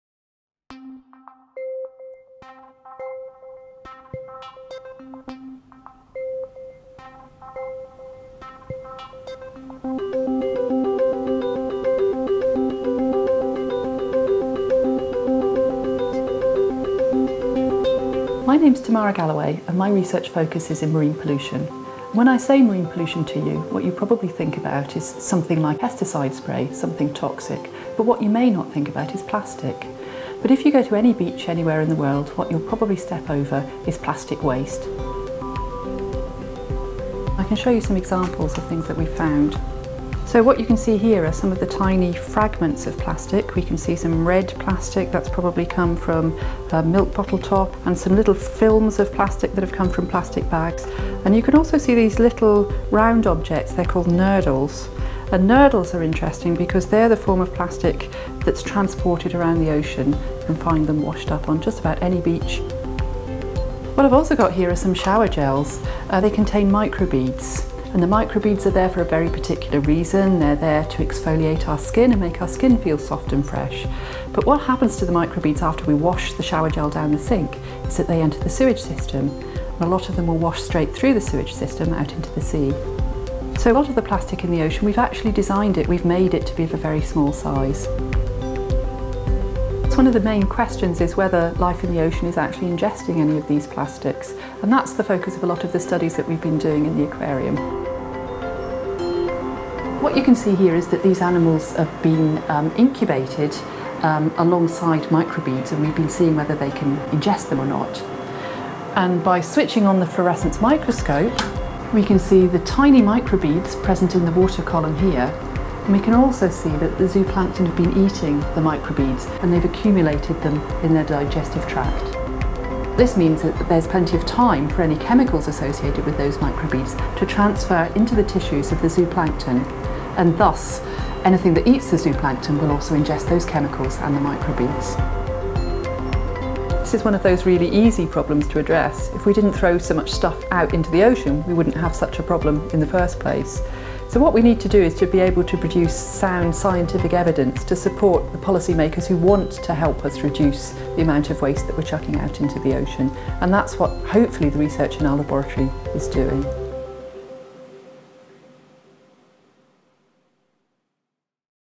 How plastics are entering the food chain
Professor Tamara Galloway of the University of Exeter explores how our plastic waste can enter the food chain through ingestion by ocean life.